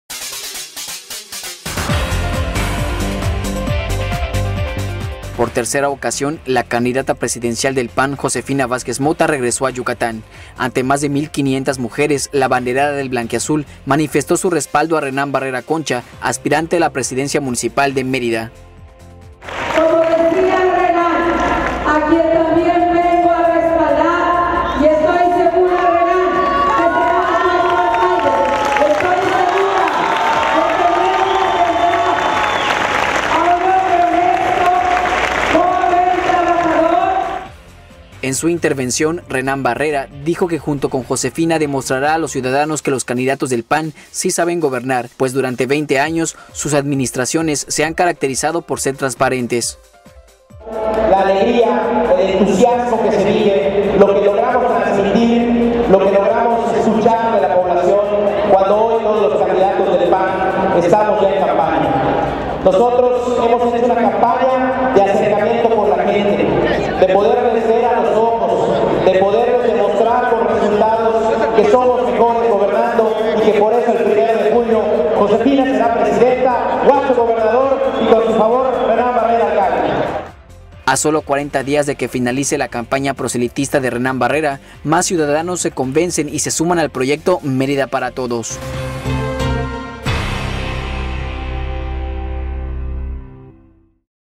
Renán Barrera Desayuno con Mujeres
Por tercera ocasión la candidata presidencial del PAN, Josefina Vázquez Mota, regresó a Yucatán. Ante más de mil 500 mujeres la abanderada del blanquiazul manifestó su respaldo a Renán Barrera Concha, aspirante a la presidencia municipal de Mérida.